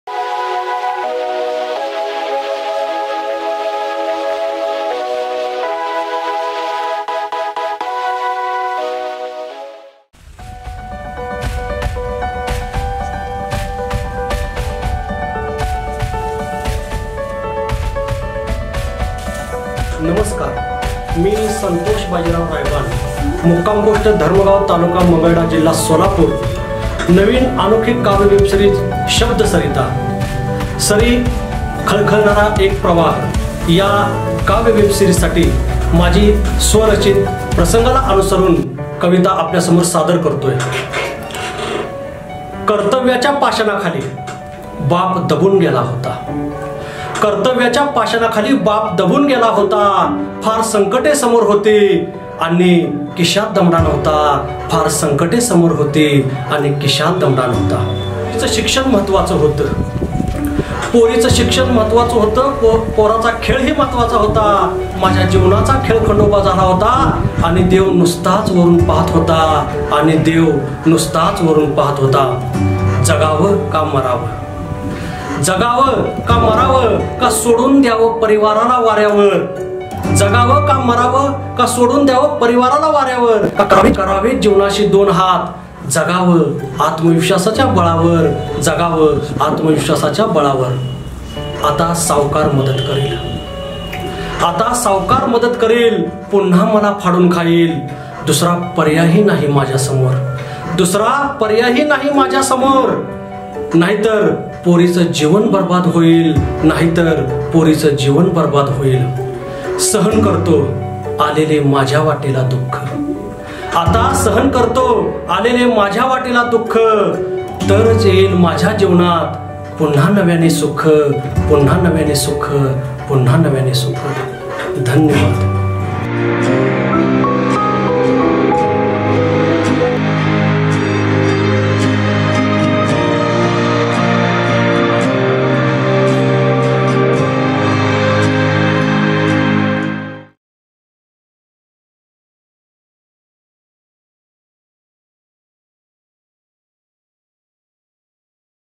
SHABD SARITA Sari...khalkhalnara ek pravah- KAVYA WEB SERIES - SEEN 1 KAVITA N0-7 SANTOSH RAYBAN
SHABD SARITA# ANOKHI NAVIN MARATHI KAVYA WEB SERIES SARI. KHALKHALNARA EK PRAVAH

 HI

MANGUDA MARATHI YOU TUBE CHANEL VAR NAVIN MARTHI WEB SERIES GHEUN YET AHE.APNALA NAKKICH AVDEL ASHI APEKSHA-SHABD SARITA...SARI khalkhalnara ek pravah
APLYA MANGUDA MARATHI# YOU TUBE CHANEL VAR PAHA
PRODUCER/WRITER/DIRECTIOR/D.O.P.:- GURUNATH GAIKWAD

CO-DIRECTOR-SIDHARTH SHINDE
CO-PRODUCER-HARIPRASAD DEOKAR

CAST-
NANA---SANTOSH RAYBAN
         PANDABA---SACHIN JADHAV

SARI RAJNANDINI
RAMYA VISHNU KOKARE
FOR MORE CONTACT
GURUNATH GAIKWAD